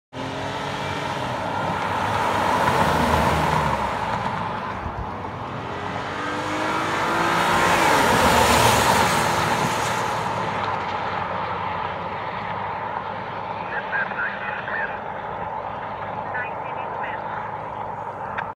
Benton County Sheriffs Office Responding
2 Benton County Sheriffs Office Units Responding for an unknown call in Benton City.